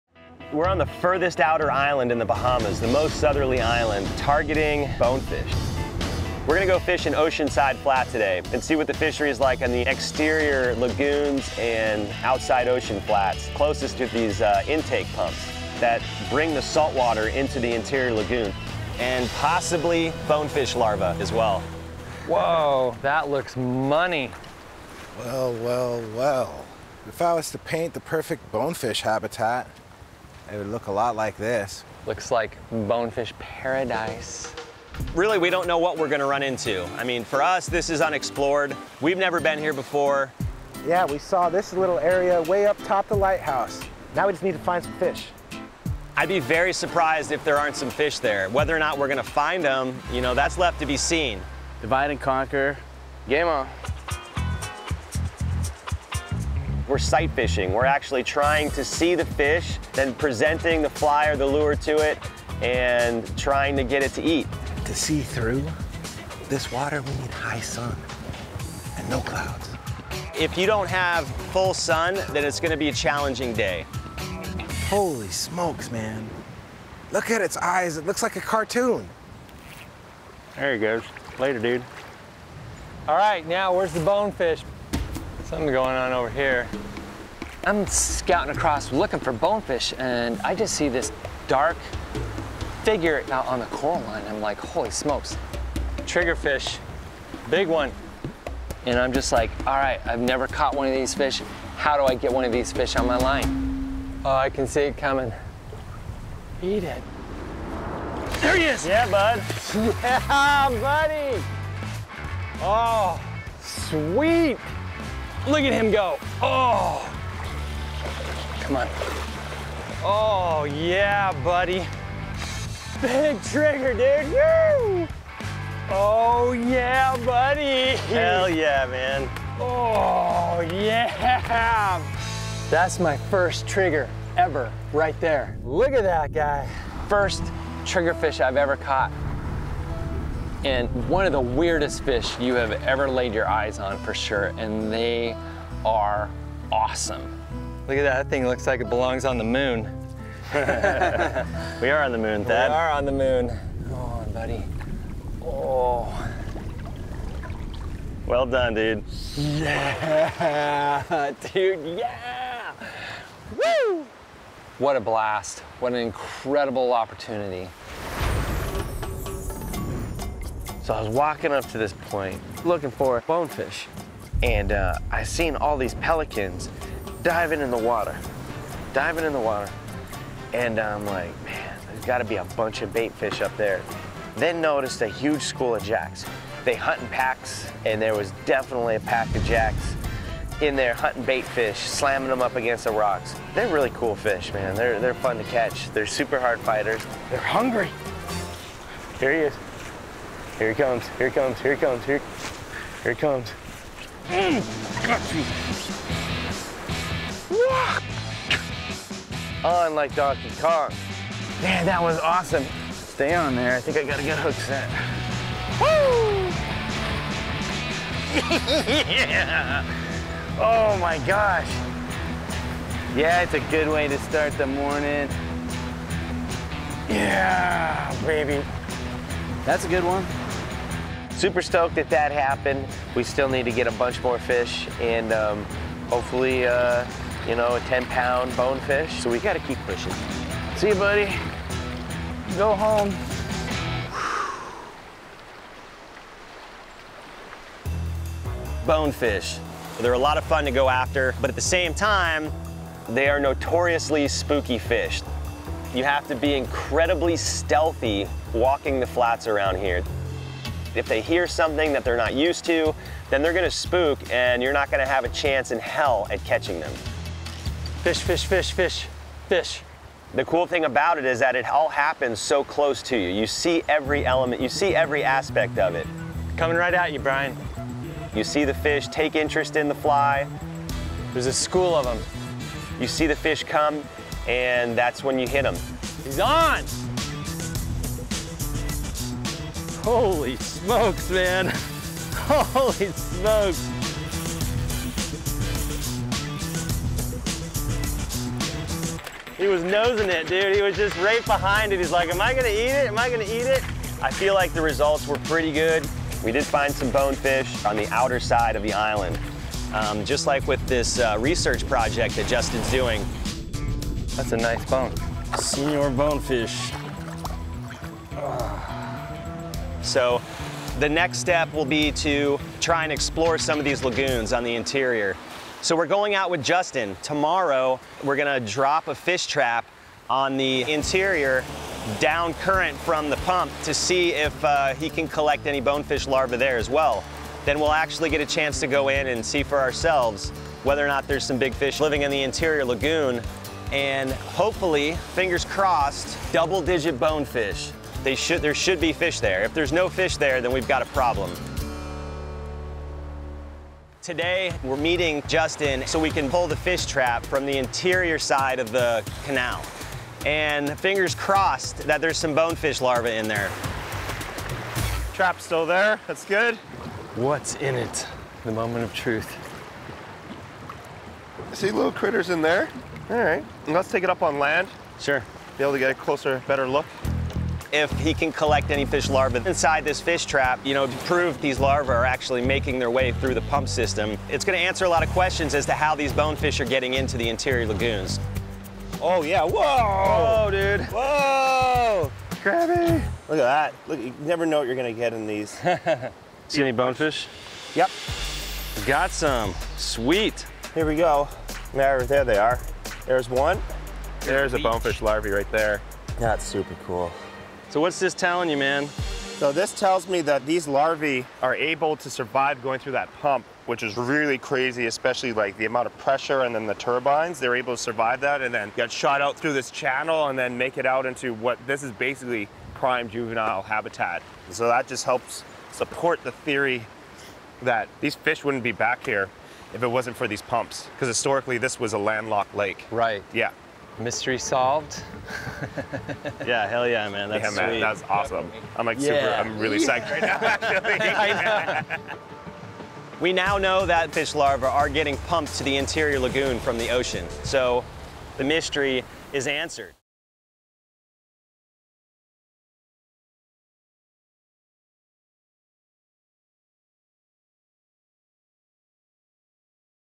Fishing For Bonefish On A Lagoon In The Bahamas | Fish Or Die
The guys are on the southernmost island of the Bahamas, searching for bonefish in the lagoons on the island. They also help find out how bonefish larvae get to the landlocked lagoons and fisheries.